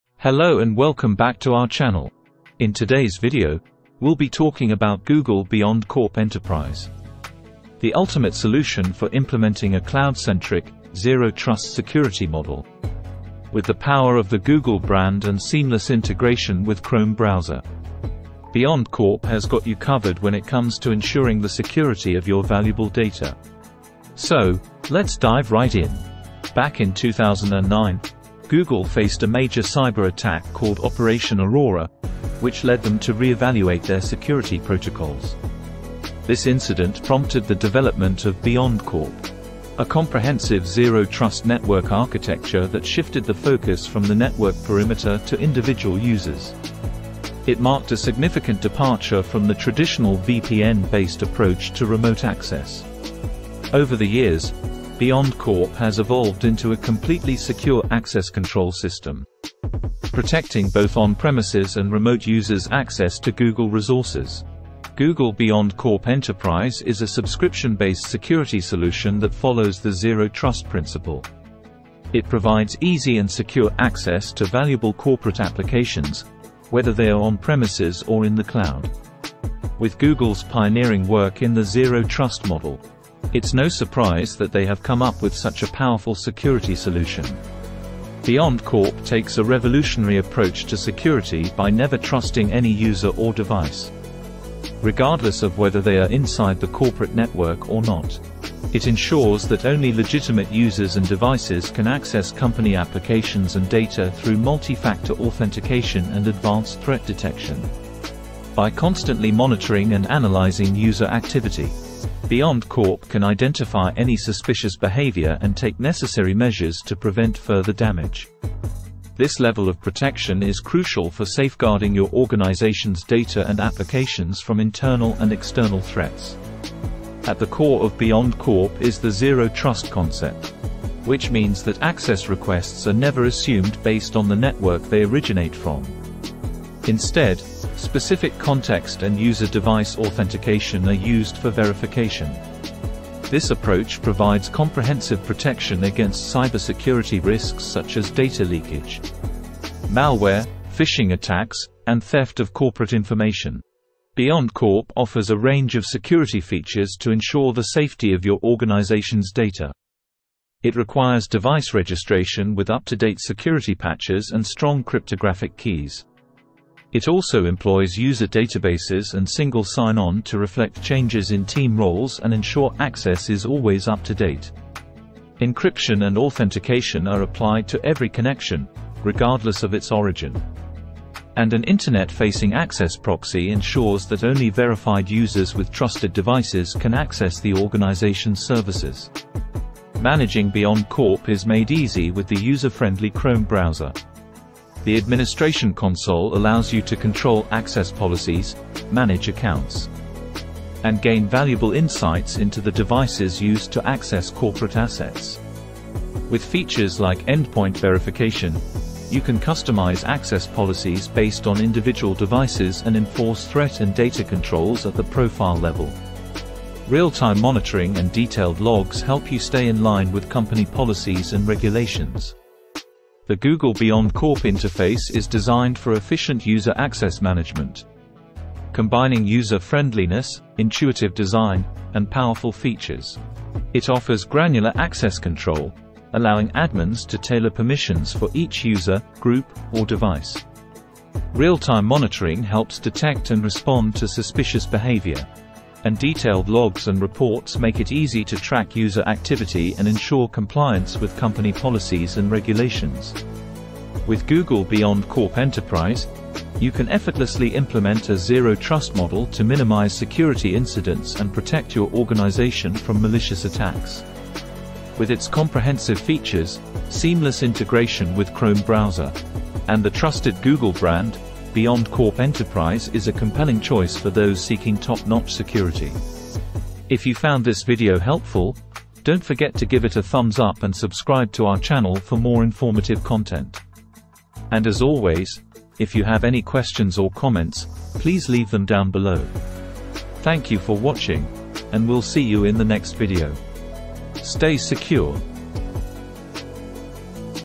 Google BeyondCorp: The Future of Enterprise Security - Full Review
Welcome to our comprehensive Google BeyondCorp Review! In this video, we explore the revolutionary security model offered by Google BeyondCorp. Join us as we discuss how this zero-trust framework can transform your organization's security posture and protect your sensitive data from unauthorized access. We'll delve into the key principles of BeyondCorp, such as device and user verification, context-aware access controls, and continuous monitoring, that eliminate the traditional reliance on VPNs and perimeter-based security. Discover how BeyondCorp enables secure access to your applications and resources, regardless of the user's location or network, through its cloud-native architecture and granular access policies. We'll also touch on the seamless integration with Google Cloud services and third-party applications, as well as the scalability and ease of implementation. Whether you're an IT professional or a business owner, this review will provide you with valuable insights to understand the benefits and considerations of adopting Google BeyondCorp for your organization's security needs.